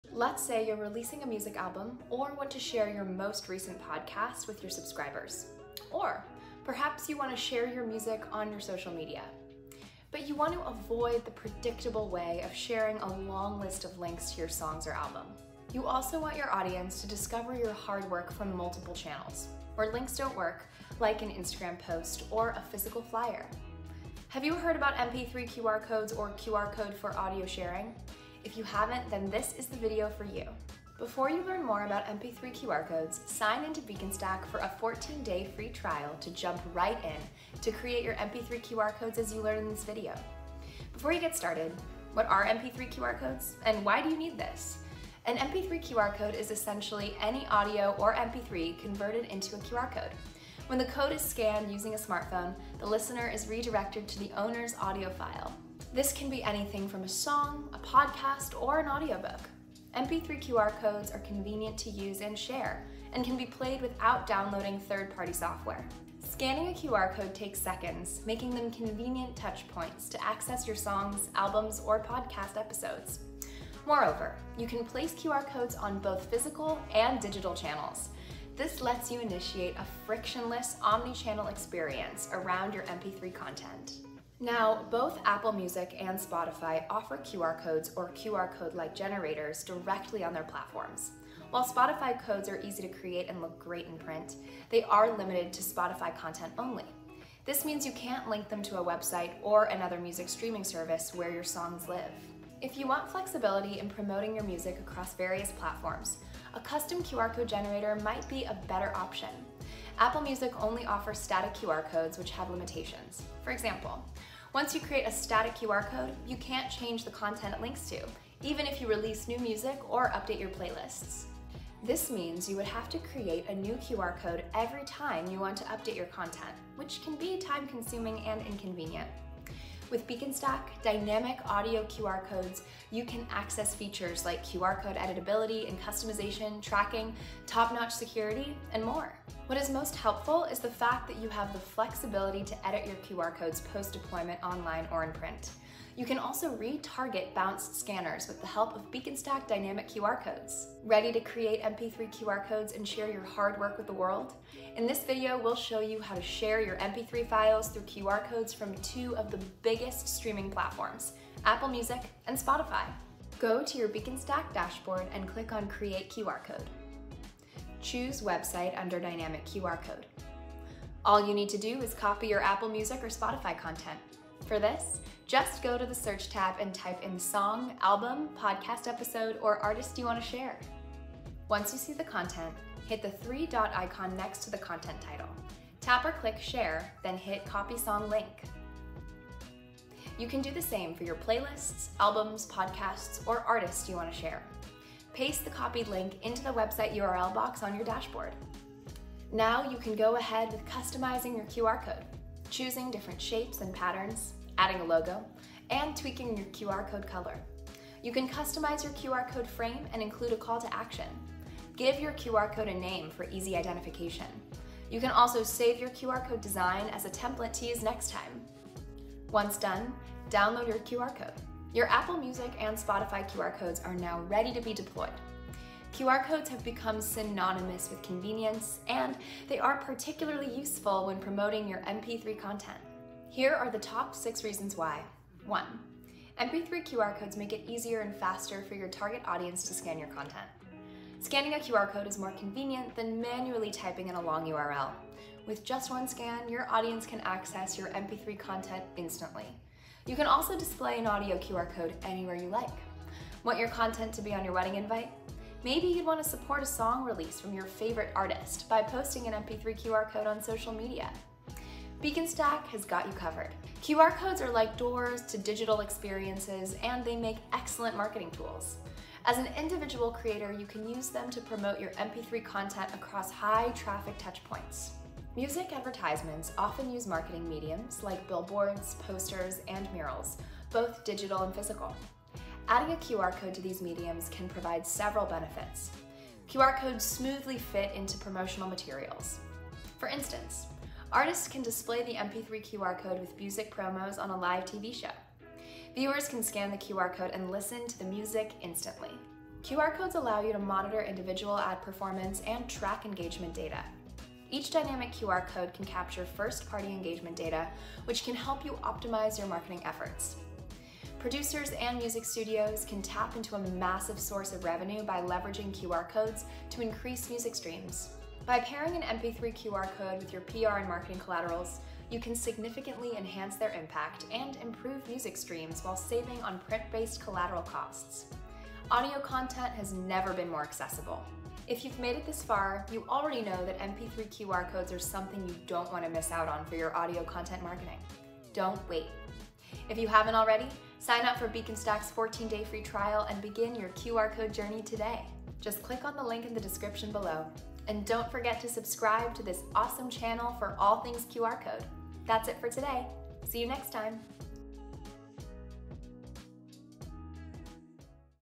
How to Create an MP3 QR Code : Unlock the future of Music Sharing 🎼
Imagine being able to share your favorite music tracks, podcasts, or audio recordings with just a simple scan.
Gone are the days of searching for links or manually transferring files. 🙋‍♀️

With MP3 QR Codes, you can easily share your audio content in an instant. 🎶

00:00 - Introduction.
00:41 - What are MP3 QR Codes?
02:35 - How to Create and Share MP3 QR Code.
03:55 - Make your Content Easier and Faster to scan.
04:10 - MP3 QR Codes are Easy to Deploy.
04:23 - Promote your Content Seamlessly.
04:59 - Monitor and Track Data.
05:11 - Create a Revenue Source.
05:39 - Sing up for a 14-Day FREE Trial.

Got questions? Put them in the comments below and we'd be happy to answer them. 😊